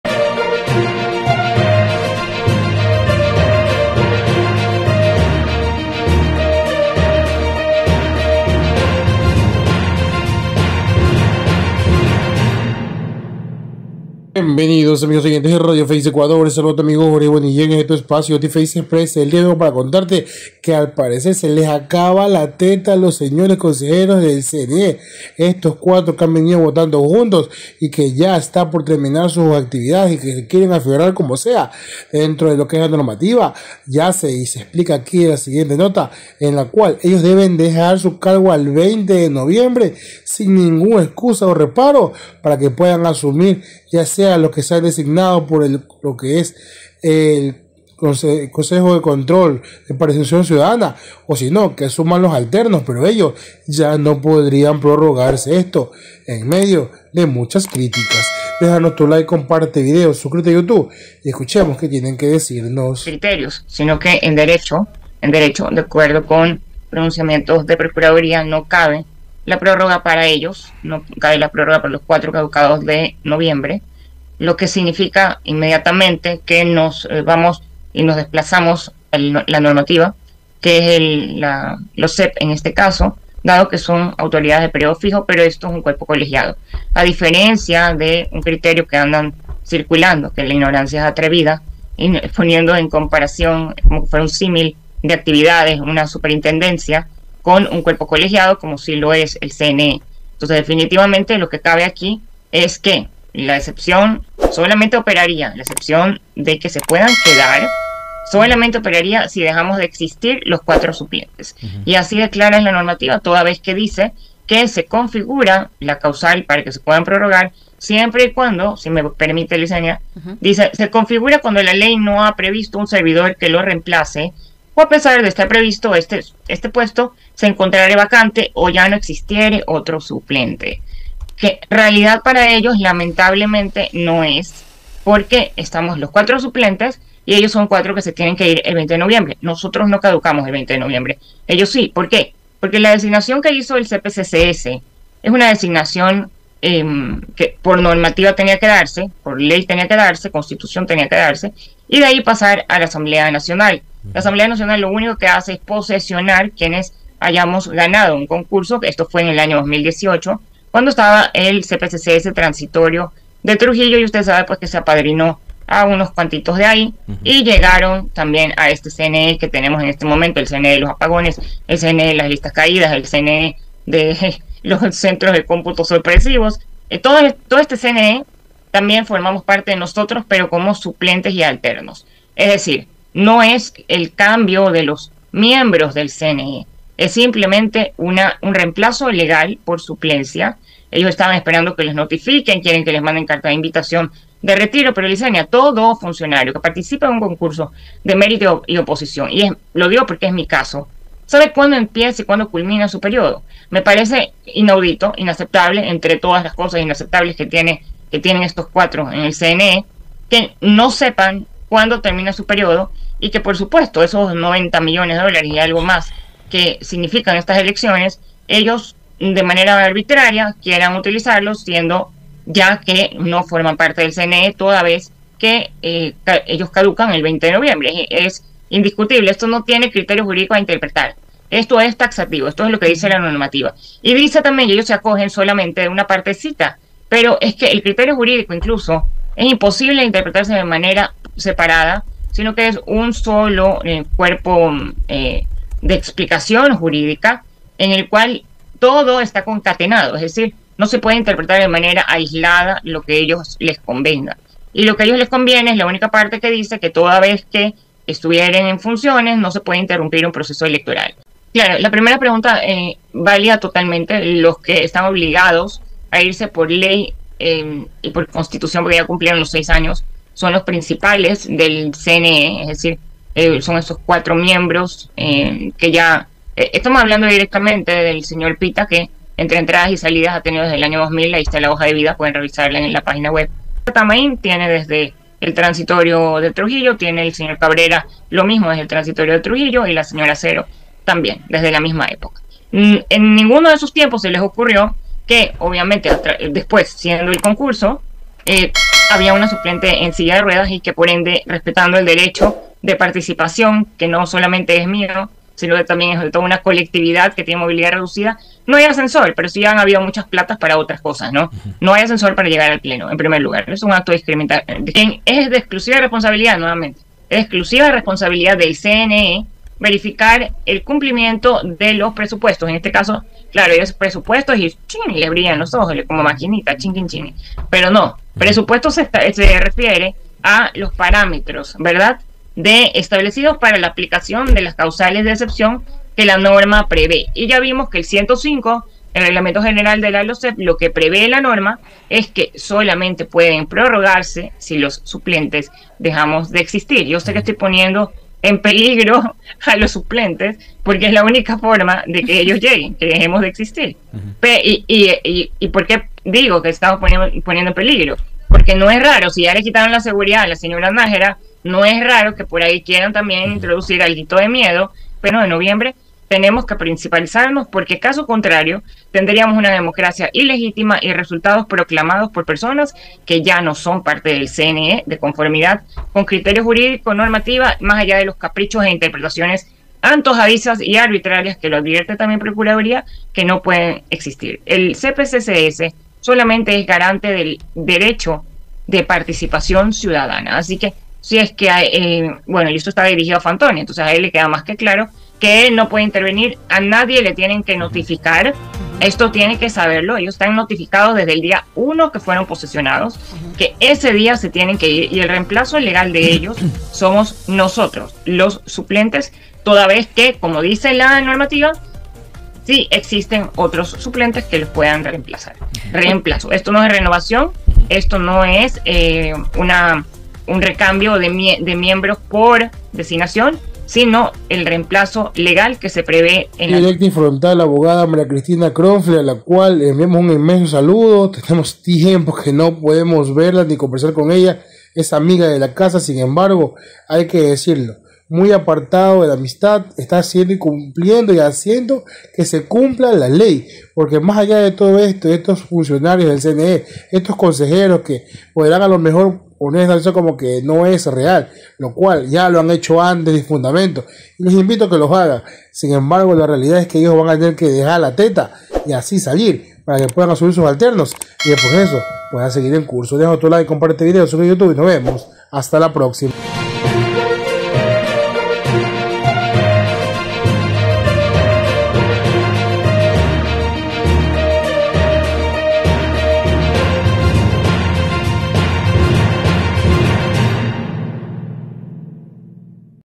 ATAMAINT Y RESTO DE CONSJEROS DEL CNE SE LES ACABO LA TE.TA DEBEN CEDER PASO A SUPLENTES NOTICIA RFE
SE LES TERMINÓ EL TIEMPO A CONSEJEROS A FIN DEL GOBIERNO DE NOBOA ATAIMAIN Y EL RESTO DE CONSJEROS DEBEN SALIR HASTA EL 20 DE NOVIEMBRE DEL 2024 SIN EXCUSAS PARA QUE ASUMA SUS ALTERNOS EL PROCESO DE ELECCIÓN 2025 NOTICIAS RFE TV 👇